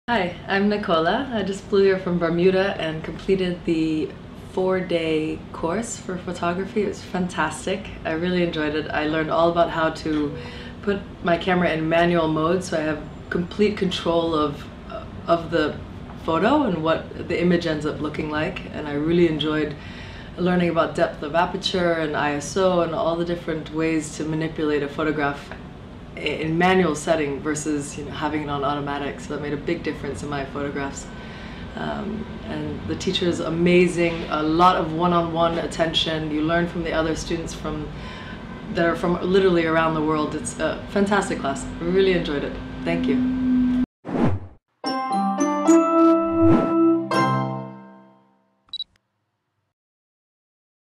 London School of Photography Full Time Photography Course Testimonial by Nicola Siso
Nicola shares with us her experience at the London School of Photography.